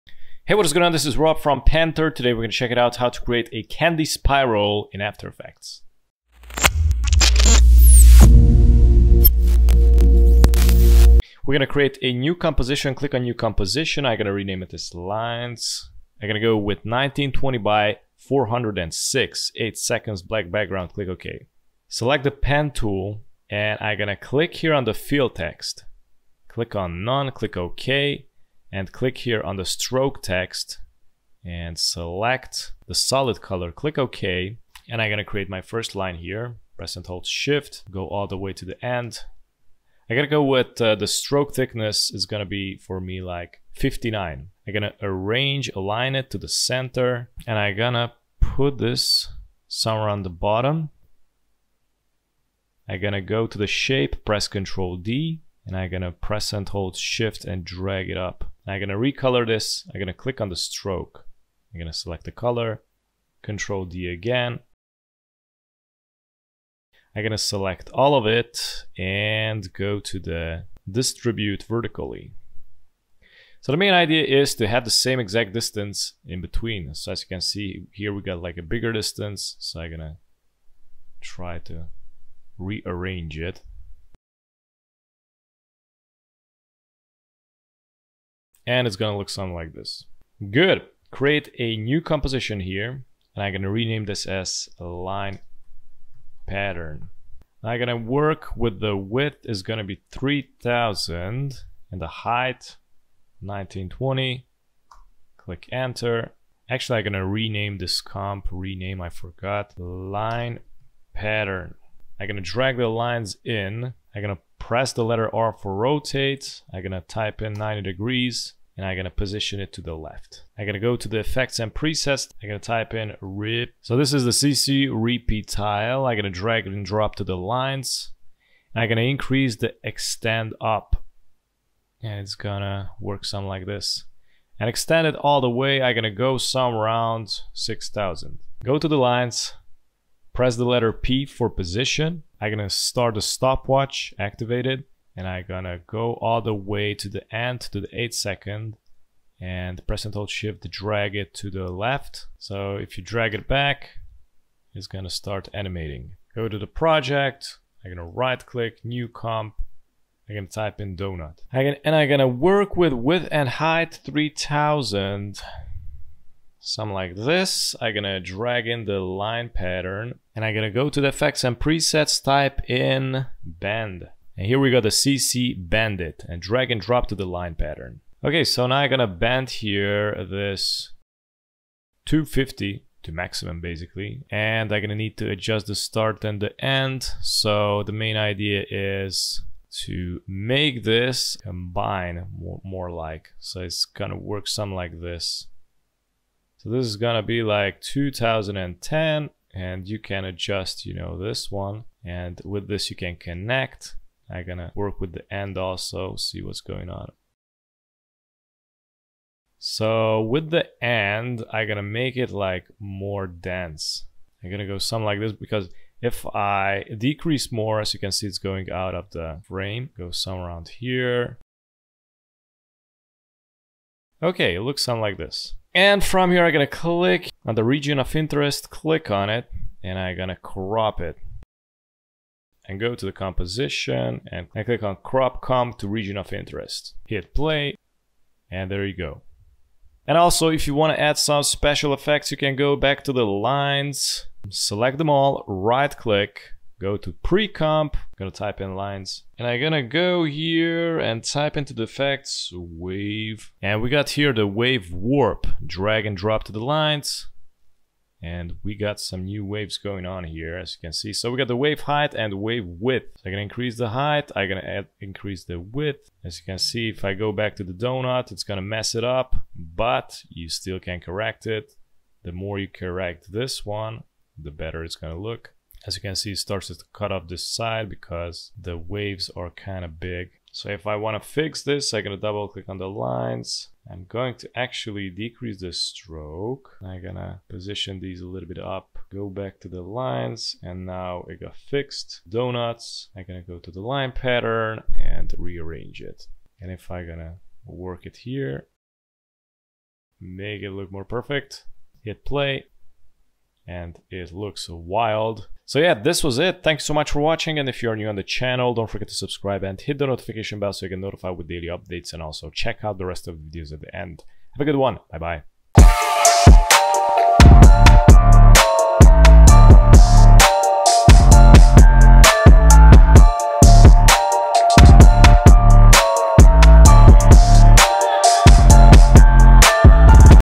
How to Make Candy Spiral in After Effects
In this tutorial you will find how to work with CC Bend and Zig Zag. The animation process has way way more potential in it and many more ways and modalities how to do these tricks and here you will find one of them.

Check out these amazing After Effects online courses where you can learn how to animate like a professional: